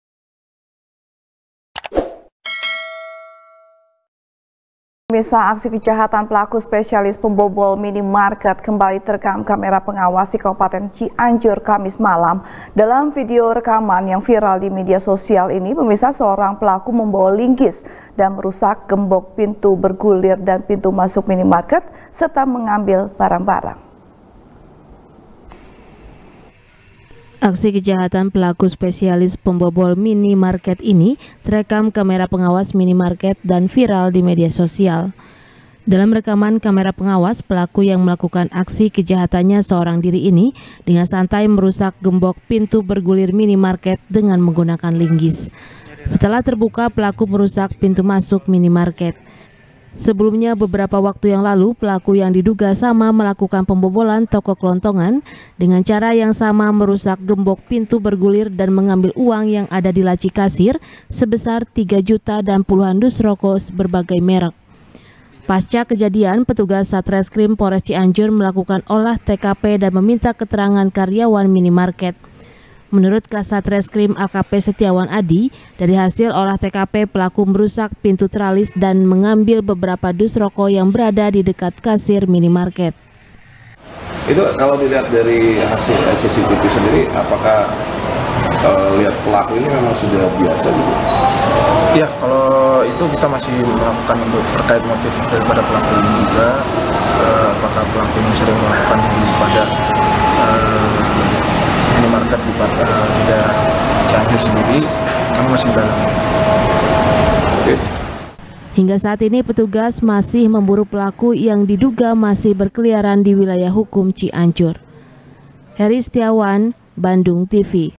Pelaku Spesialis Pembobol Minimarket Terekam Kamera Pengawas | SBR | BANDUNGTV
SBR | BANDUNGTV | FARHAT MUMTAZ | Aksi Kejahatan Pelaku Spesialis Pembobol  Mini Market, Kembali Terekam Kamera Pengawas,Di Kabupaten Cianjur , Kamis Malam, Dalam Video Rekaman Yang Viral Di Media Sosial Ini, Seorang Pelaku  Membawa Linggis, Dan Merusak Gembok Pintu Bergulir Dan Pintu Masuk Mini Market Serta Mengambil Barang-Barang .